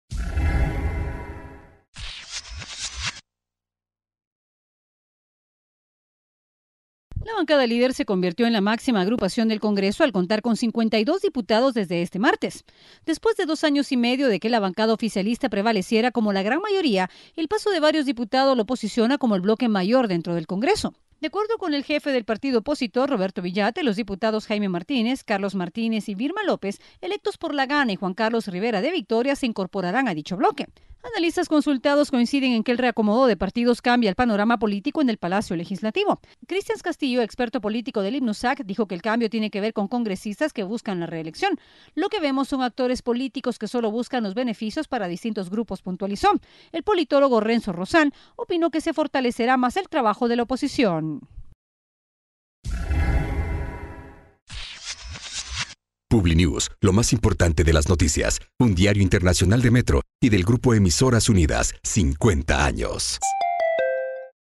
Líder es mayoría en el Congreso
La bancada Líder se convirtió  en  la máxima agrupación del Congreso al contar con 52 diputados desde este martes después de dos años y medio de que la bancada oficialista prevaleciera como la gran mayoría